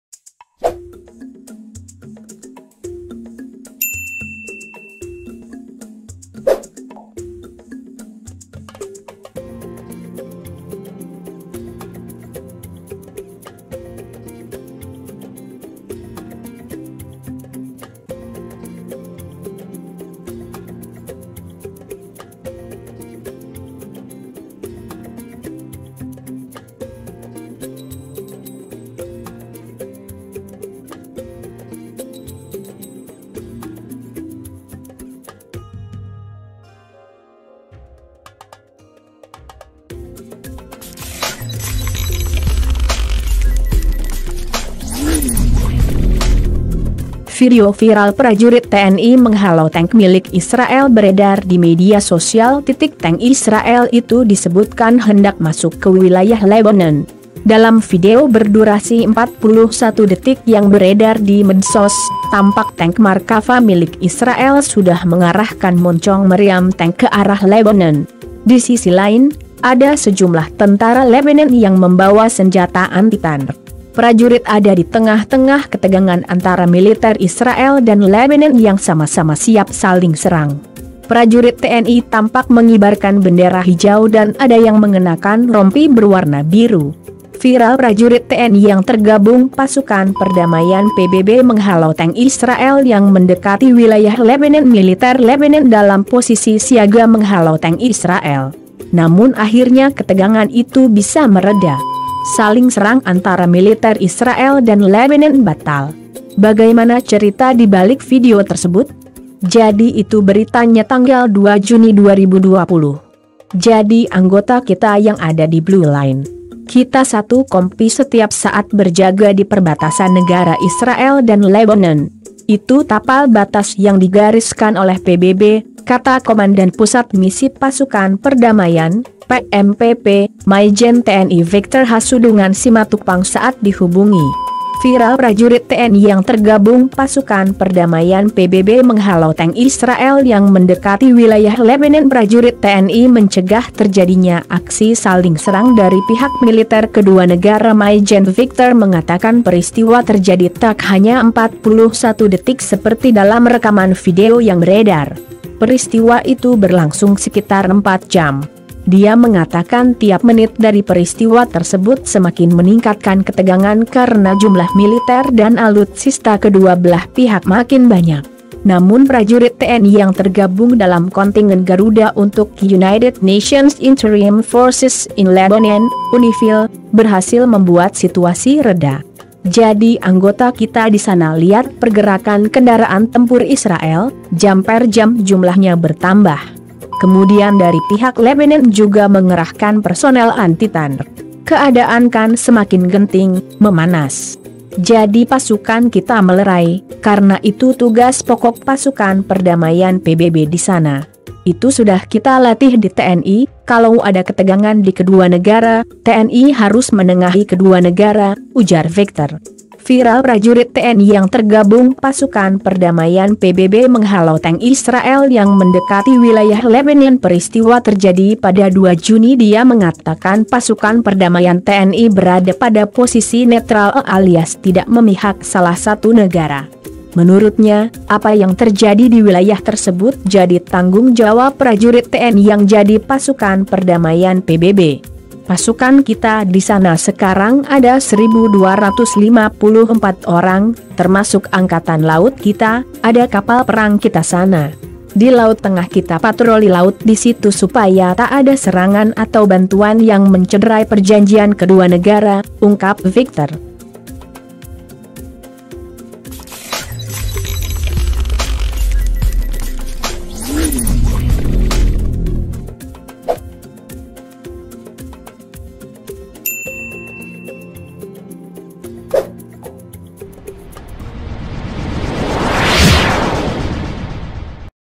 VIRAL!! Aksi TNI hadang tank israel di perbatasan Lebanon
Nama TNI kembali mendunia setelah aksi penghentian perang antara Israel vs Lebanon baru-baru ini.

Bahkan prajuri TNI dengan gagah berani hadang kedua rombongan pasukan baik dari Lebanon maupun Israel.

Aksi prajurit TNI yang tergabung dalam Kontingen Garuda XXIII-N/United Nations Interim Forces in Lebanon (UNIFIL) tengah menjadi sorotan.

Prajurit TNI yang bertugas menjaga perdamaian di perbatasan antara Israel dan Lebanon, berhasil menggagalkan peperangan antara kedua negara tersebut